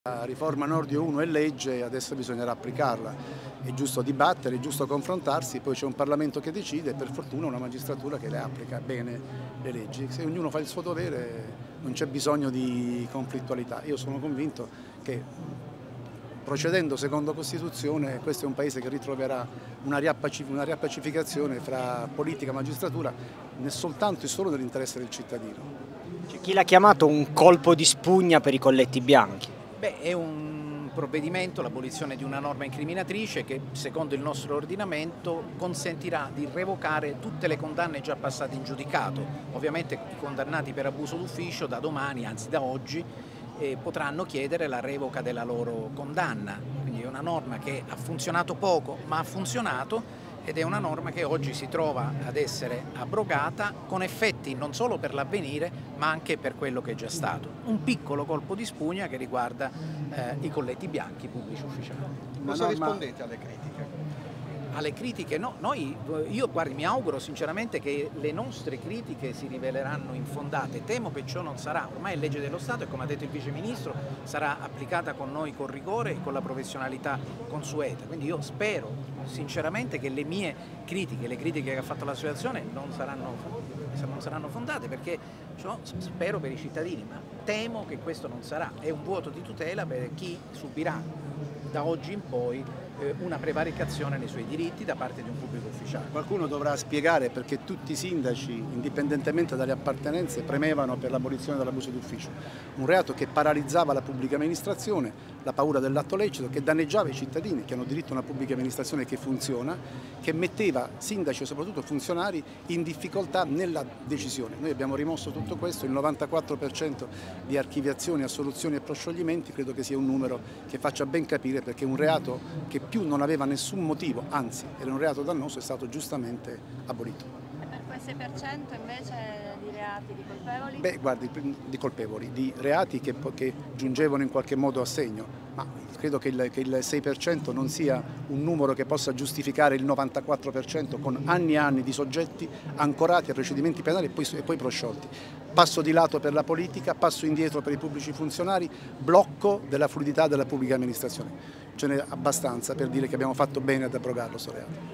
Intervista a Giuseppe Santalucia e Francesco Paolo Sisto, 12/07, XXIII edizione il Libro Possibile
La terza giornata della XXIII edizione del festival Il Libro Possibile, il 13 luglio 2024 a Polignano a Mare. Tra gli ospiti: Umberto Galimberti, Francesca Santolini, Giovanni Grasso, Massimo Franco, Vincenzo Schettini, Pierpaolo Spollon, Karima Moual.

Si parla di ecologia, giustizia, intelligenza artificiale, e tanto altro.

Video dall'Ufficio Stampa Il Libro Possibile.